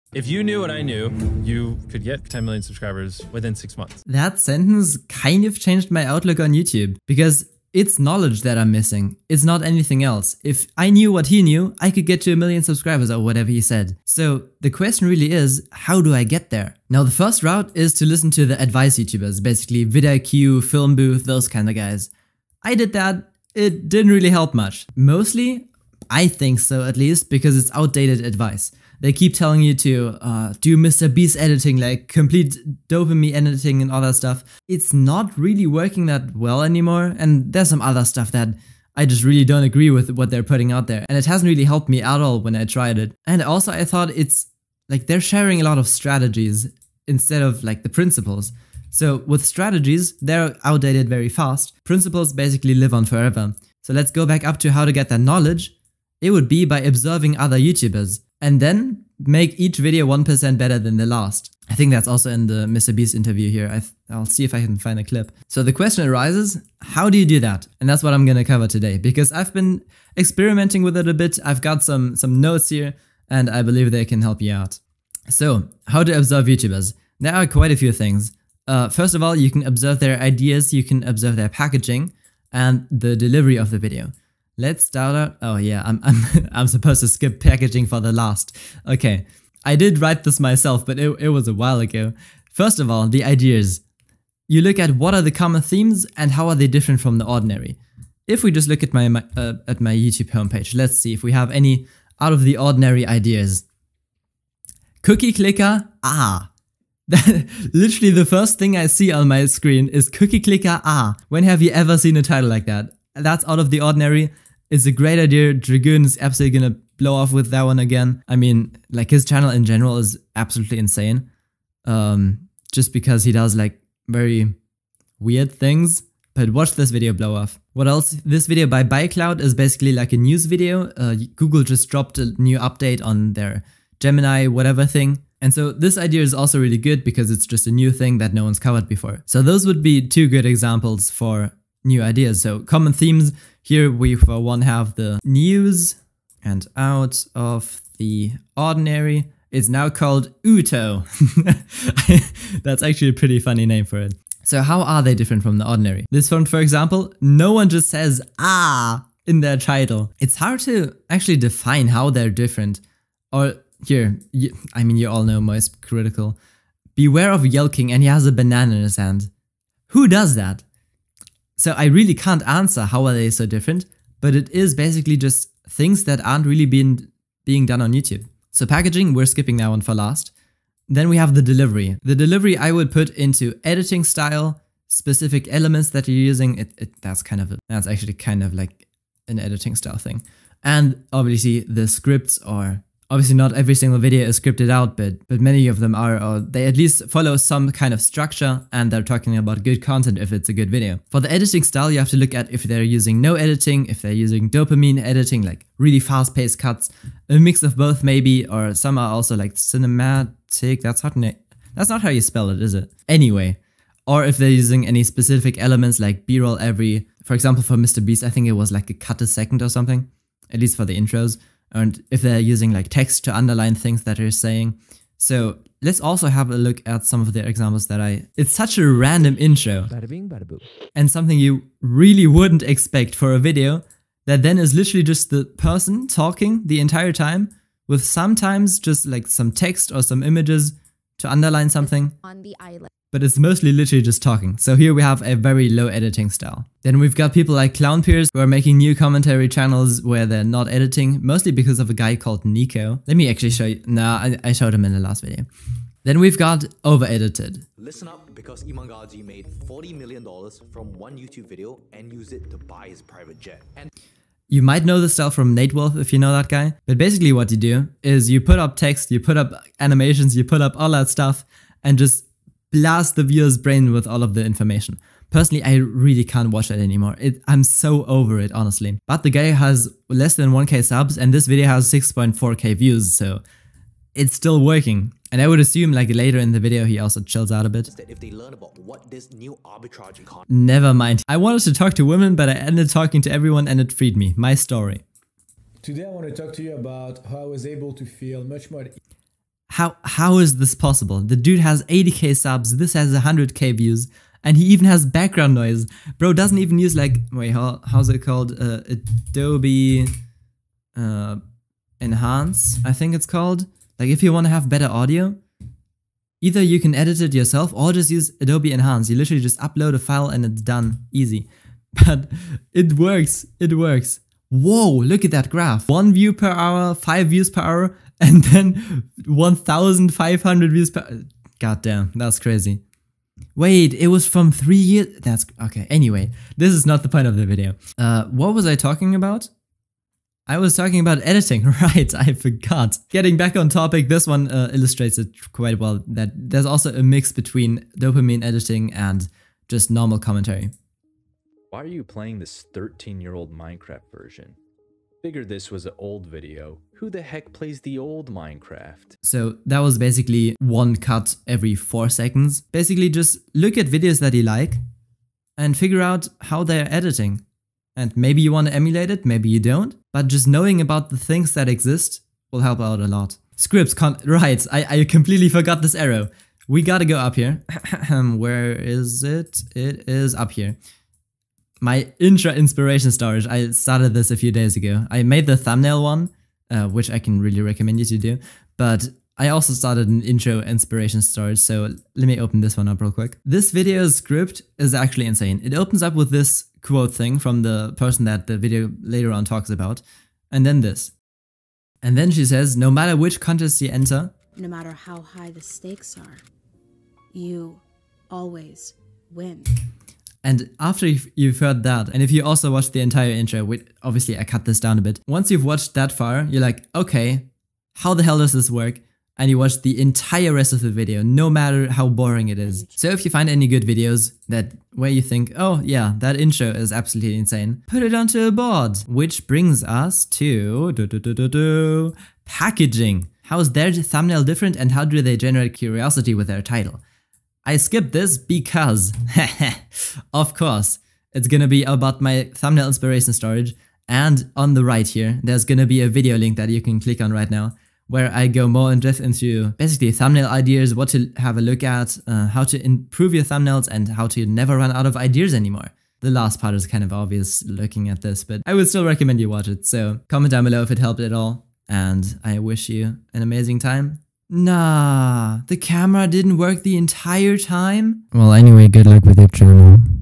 This Secret Changed My Channel
My NEW community for skool owners & entrepreneurs (it's free) This YouTube Secret changed my channel forever. I finally stopped watching VidIQ all the time and focused on what's actually important.

Here's how to never run out of video ideas anymore, how to improve storytelling, thumbnails, and so much more!